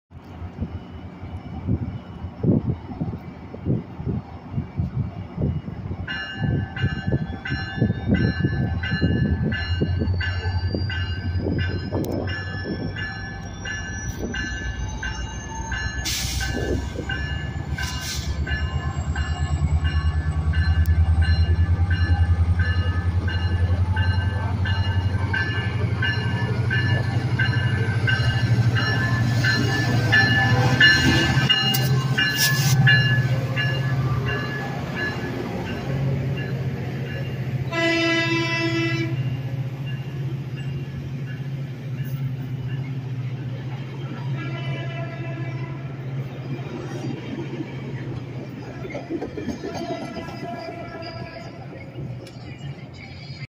Metrolink Train 312 to San Bernardino from Fontana SCAX MP36 895 & Cab Car 676 2/13/2024
Metrolink Train 312 At Fontana Station
1. Metrolink Train 312 to San Bernardino-Downtown from Claremont SCAX MP36 895 & Hyundai Rotem Cab Car 676